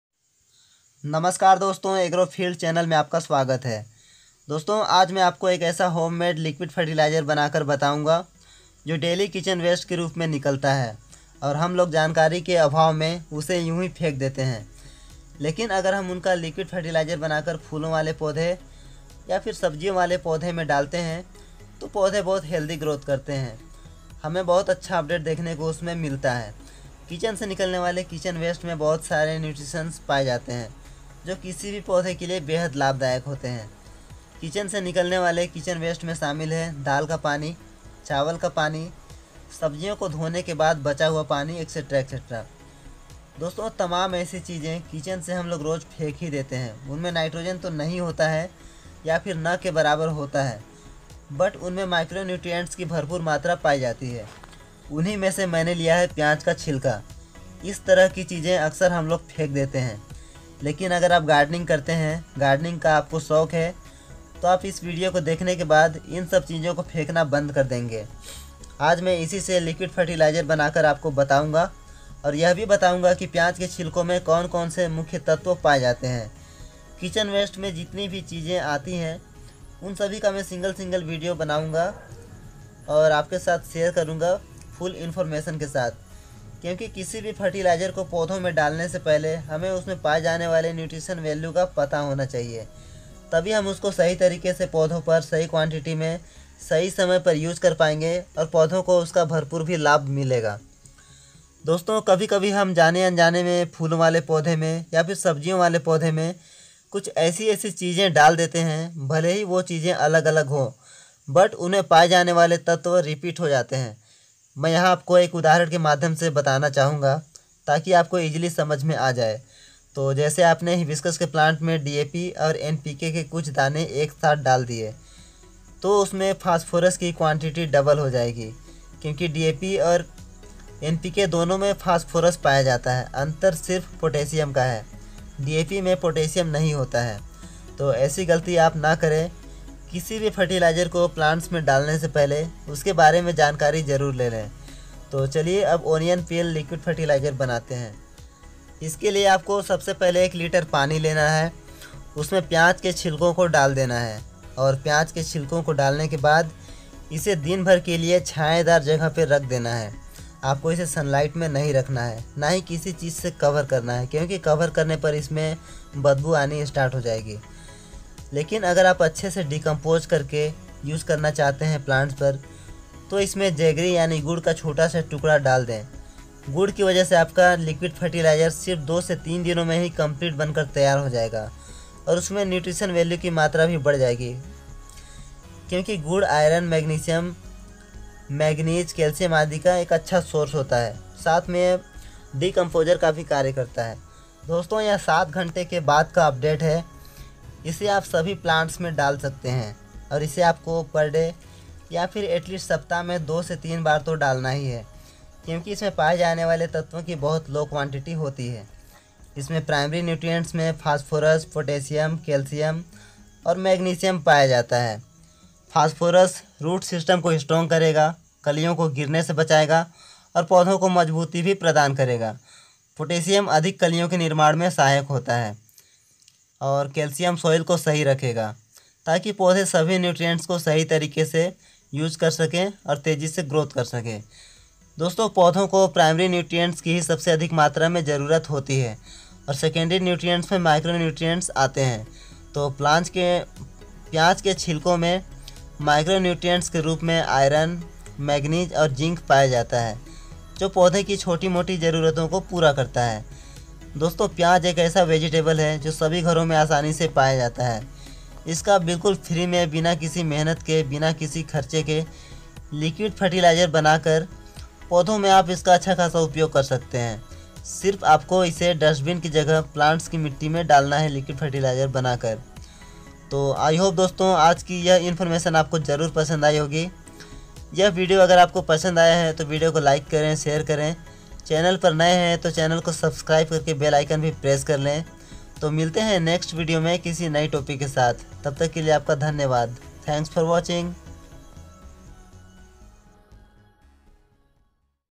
फूलों से भर जायेंगे फूलों वाले पौधे इसको डालने से/onion peel fertilizer for all plants
नमस्कार दोस्तों Agro Field चैनल में आपका स्वागत है इस वीडियो में मैंनें प्याज के छिलको का सभी पौधों पर होने वाले फायदे के बारे में बताया है। घंटी वाले बटन को भी दबायें।
धन्यवाद।
कमाल का असर प्याज के छिलको का सभी पौधों पर/onion peel fertilizer for all plants